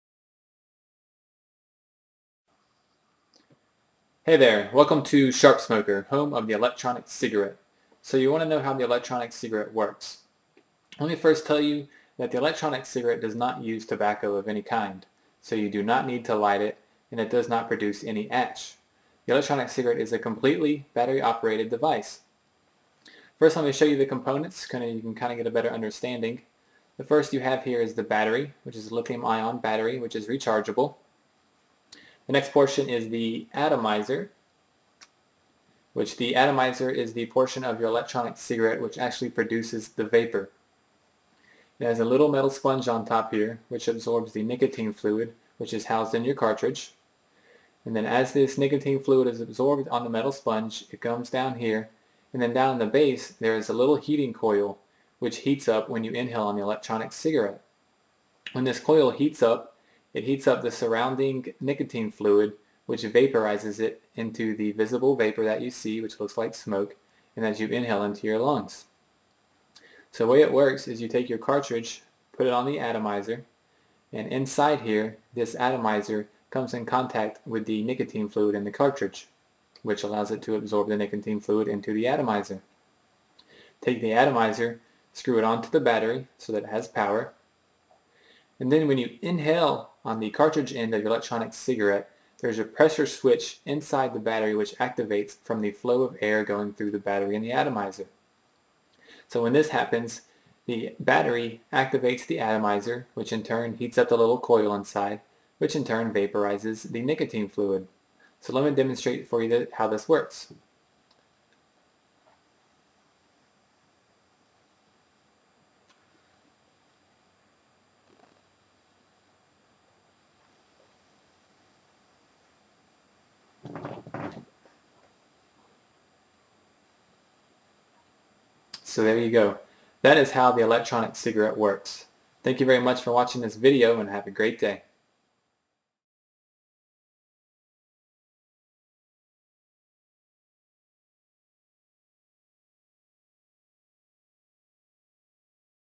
How the Electronic Cigarette Works
- In this video I demonstrate and explain how the Electronic Cigarette works.  I am using an M402 Electronic Cigarette in this video to show the three different parts of the e-cig: the battery, atomizer, and nicotine cartridge.  To learn more, please visit my